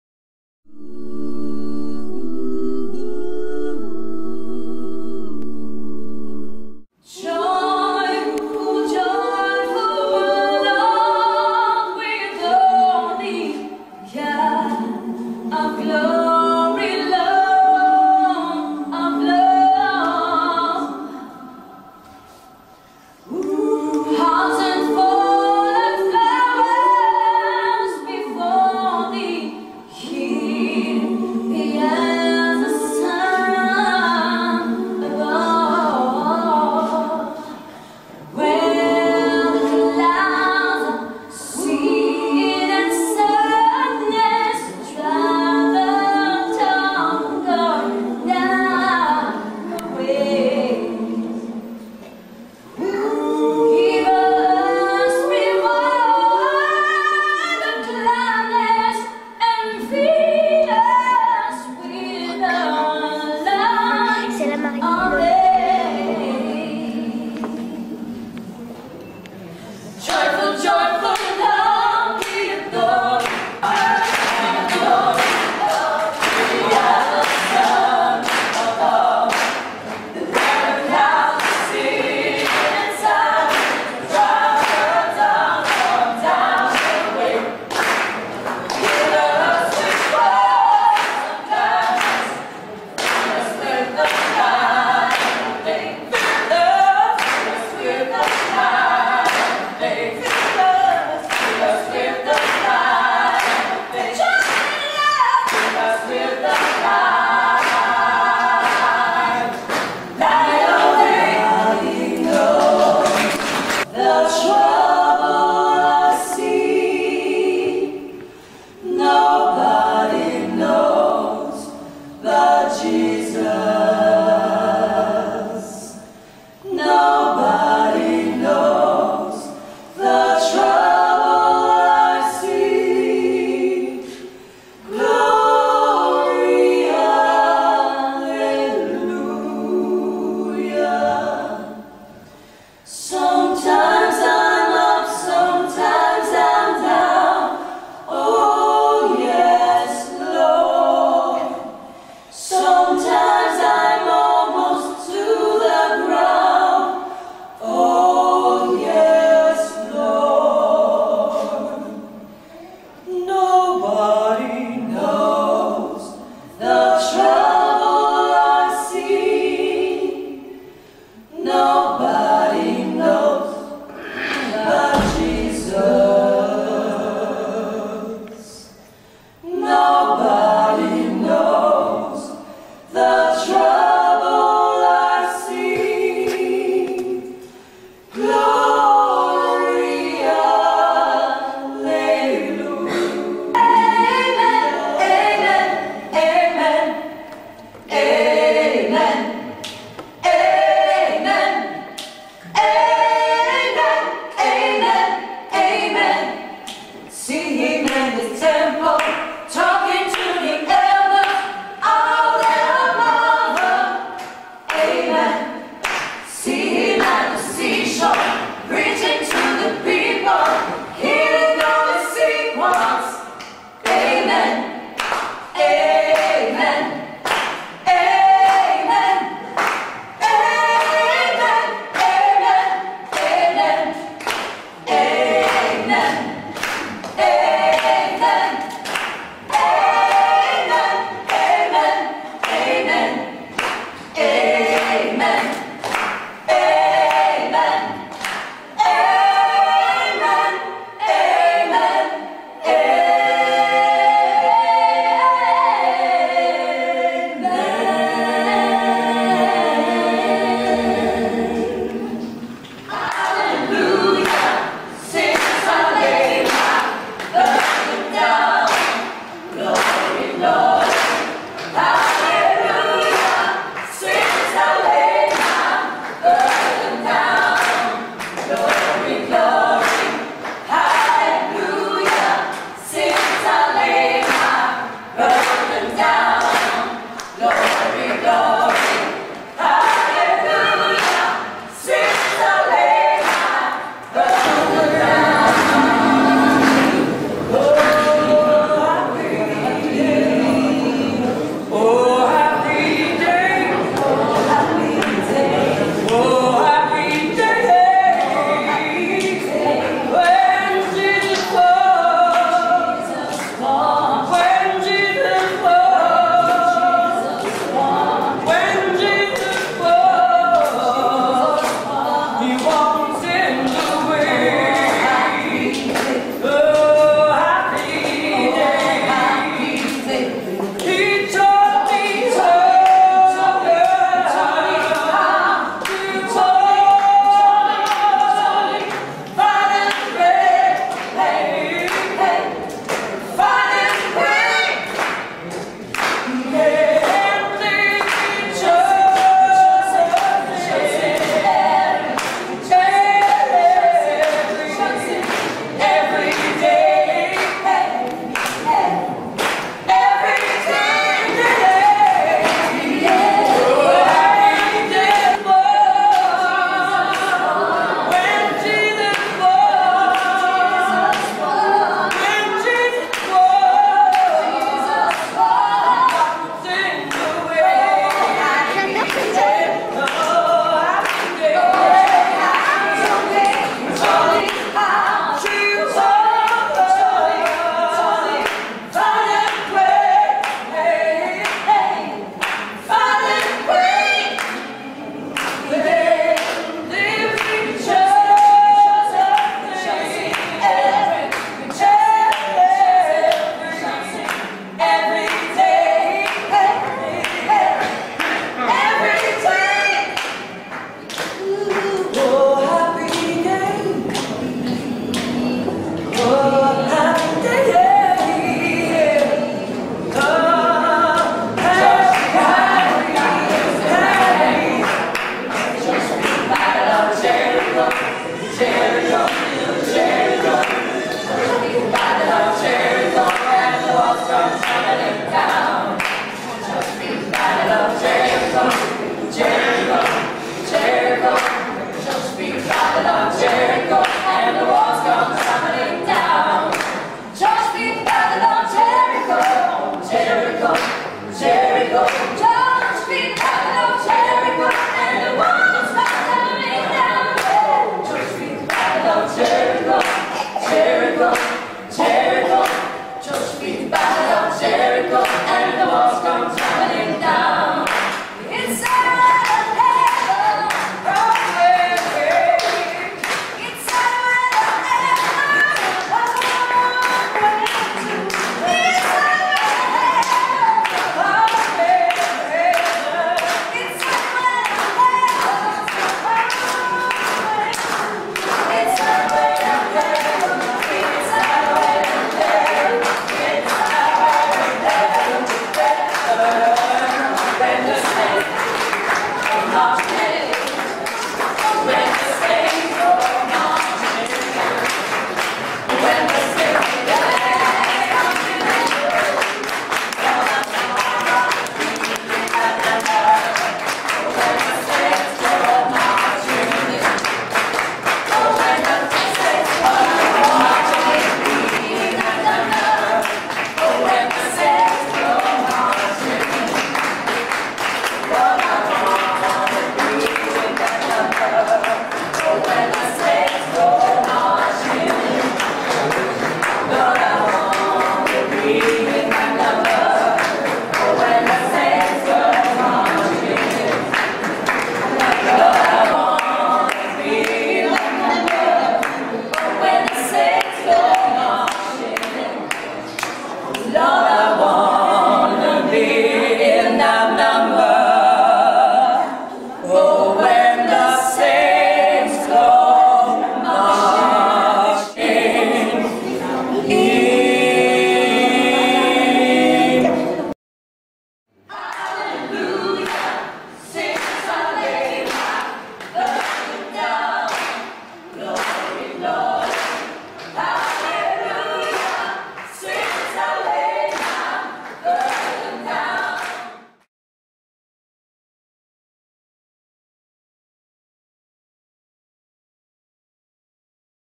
PURE VOICES - CEREMONIE MARIAGE
Vidéo live du trio gospel féminin Pure Voices (ex-Pure Gospel) exclusivement a cappella, enregistrée en juillet 2008. Arrangements écrits par Véronique Mavros - SITE WEB ou